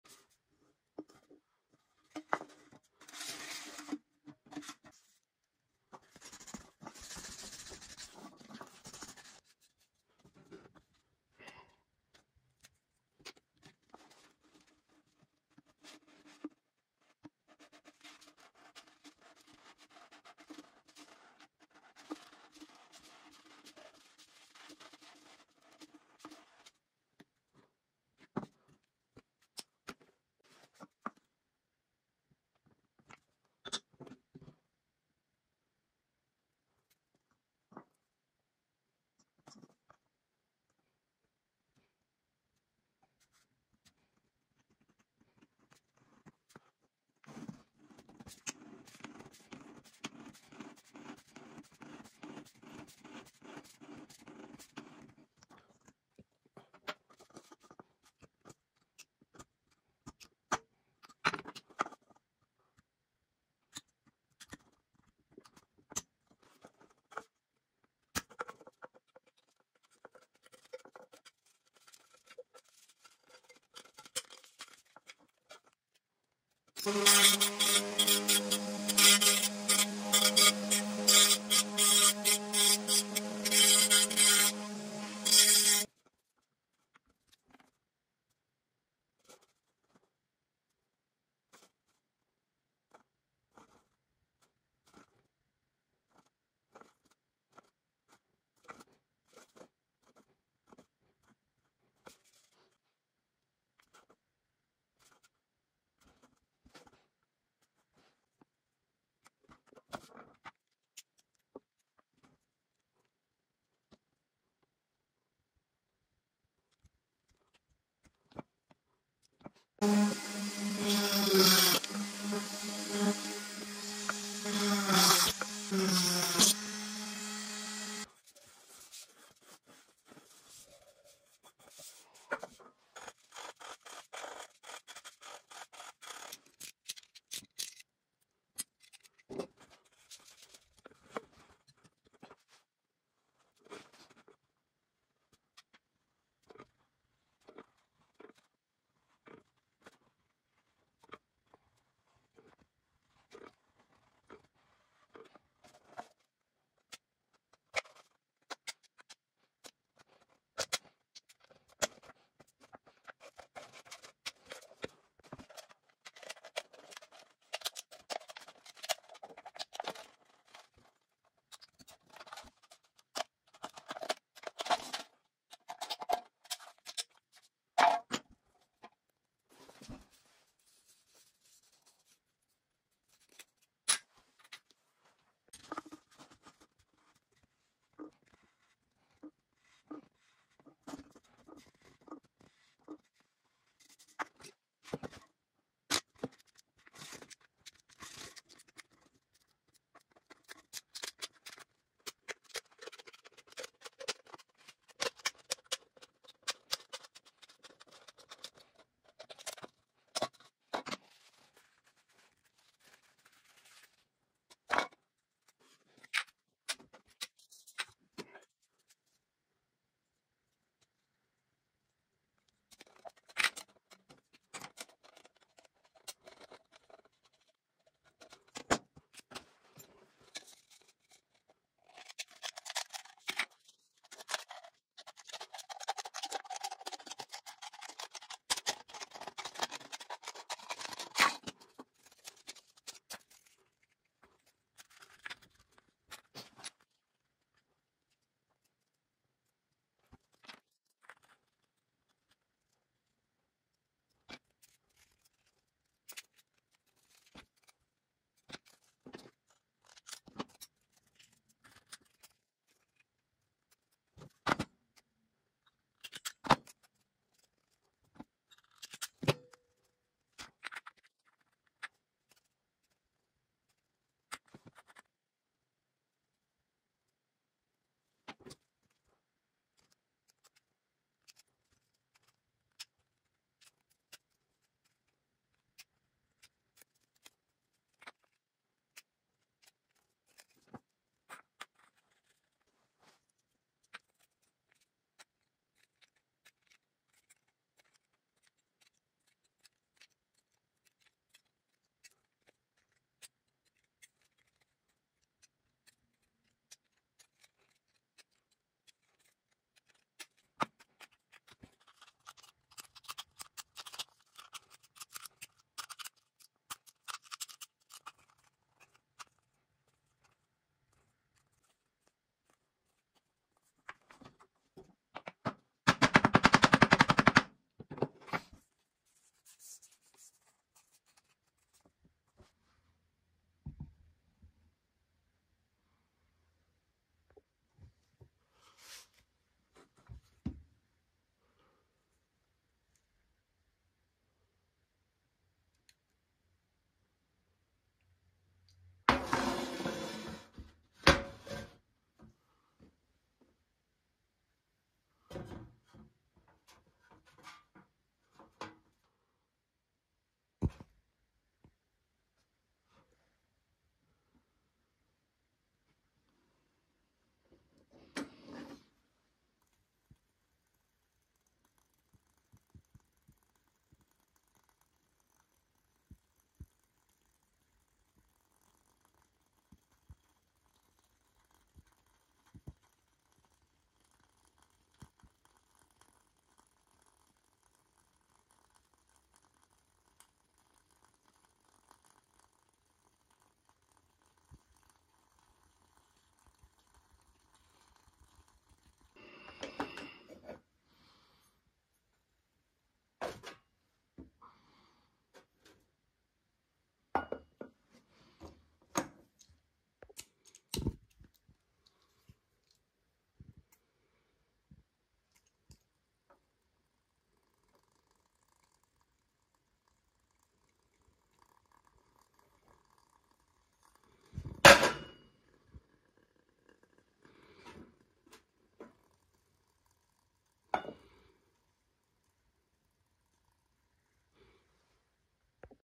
Making A Tiny Alchol Stove
Selam Dostlar
Bu videomuzda içecek kutusundan minik alkol sobası yapımını izleyeceksiniz.
İyi Seyirler👍